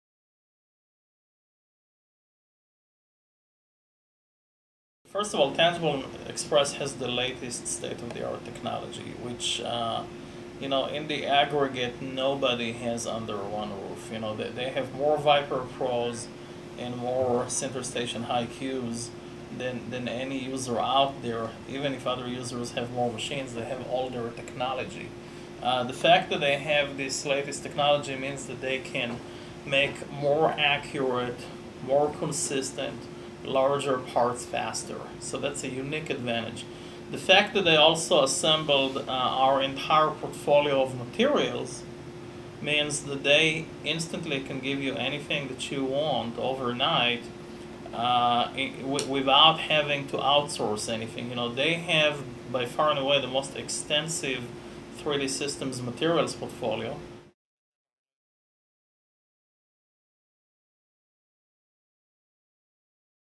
Abe Reichental, President and CEO of 3D Systems talks about
Abe Reichental, President and CEO of 3D Systems talks about Tangible Expresses capabilities.